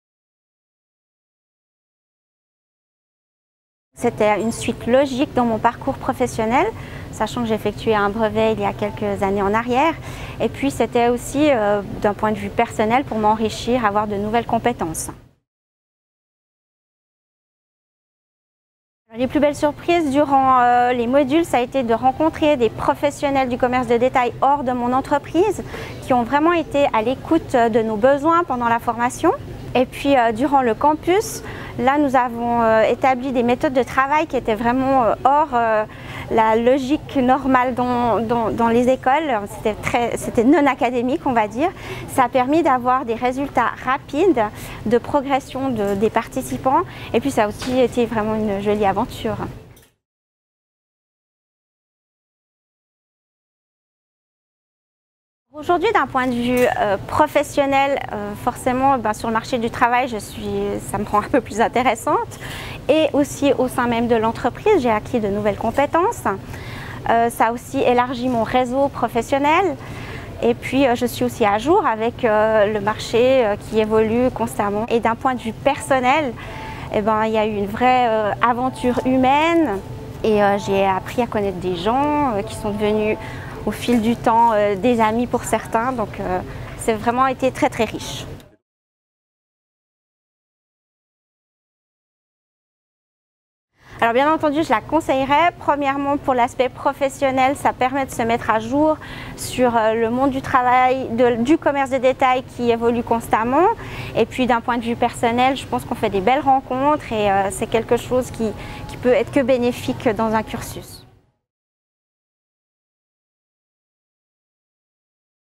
Magaly Weiler, Gérante du M-Parc Micasa et SportXX
Magaly Weiler est Gérante du M-Parc Micasa et SportXX. Elle elle répond aux questions concernant la formation continue „Manager du commerce de détail“.